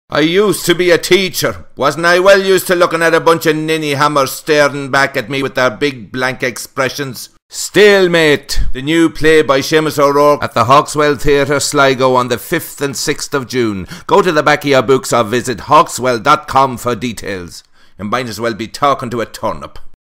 Stalemate
Seamus O'Rourke takes to the boards in a cast of four in his brand new comedy in Sligo.